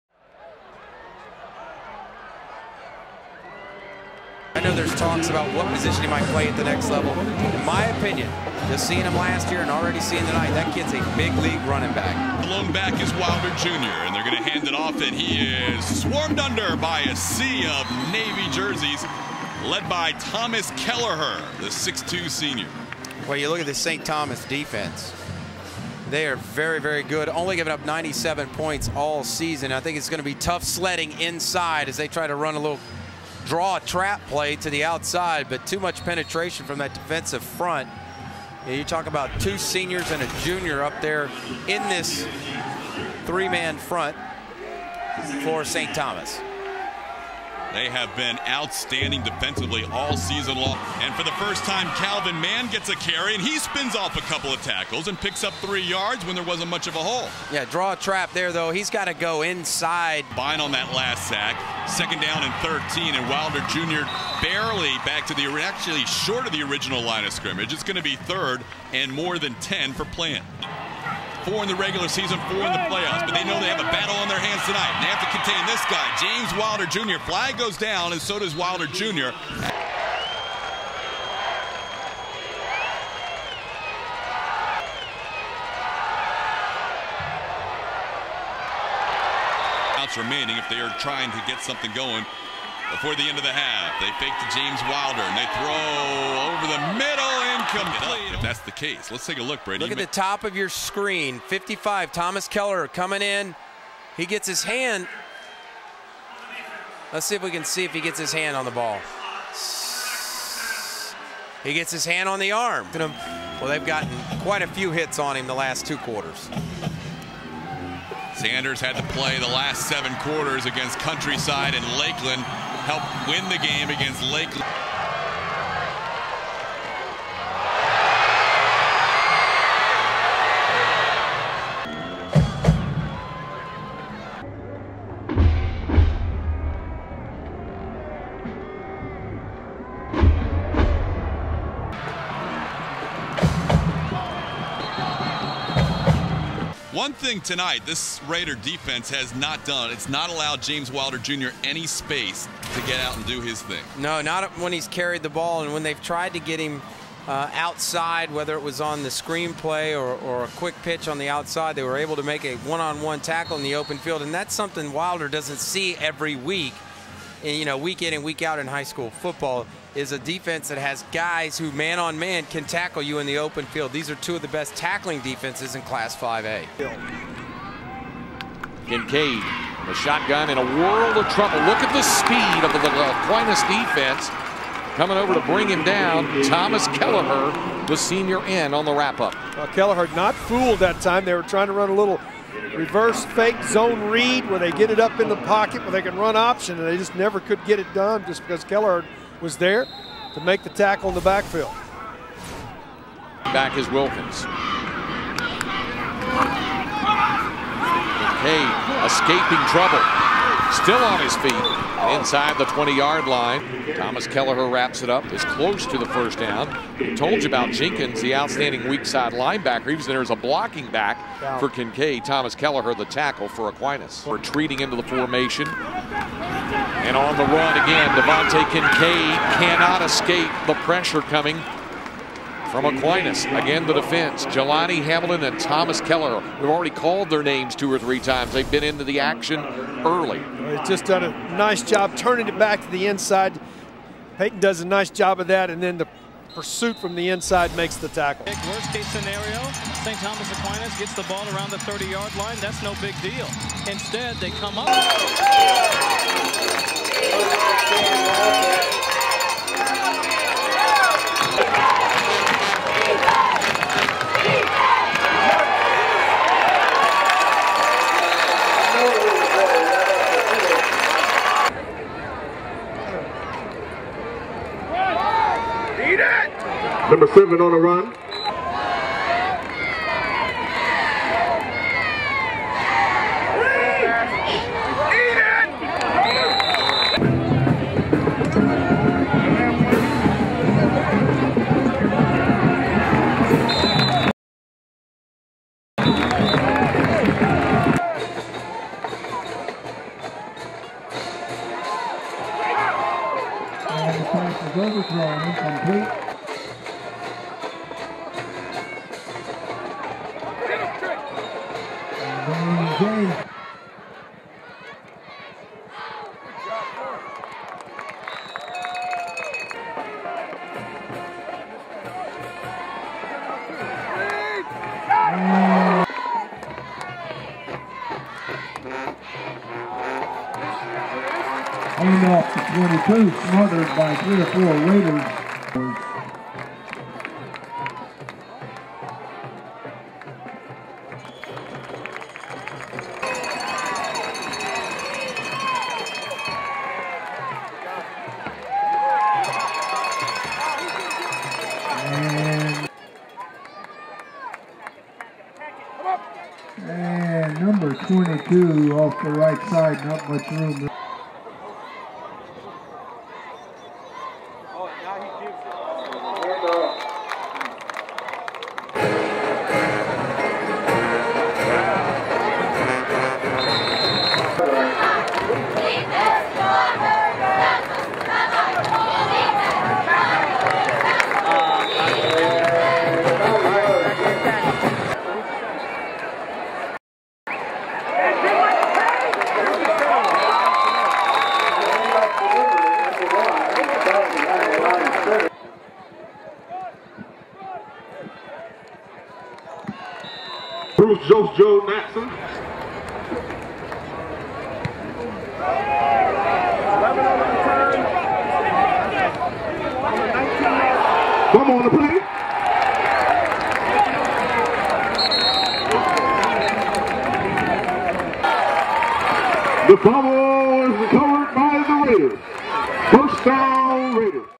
Thomas Kelleher #55 DT/DB -2010 Highlights
Thomas Kelleher DL #55
6'3"
221lbs
Class of 2011
St Thomas Aquinas
Ft. Lauderdale
2010 National Champions
2010 State Champions

31 tackles / 3 Assists
11.5 for loss / 45yds
3 sacks for 22yds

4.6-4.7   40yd
Squats 425 lbs
Bench   285
One arm cleans 135 lbs 15 reps each arm
additional conditioning-Wrestles, Boxes MMA, Pa lades, Etc.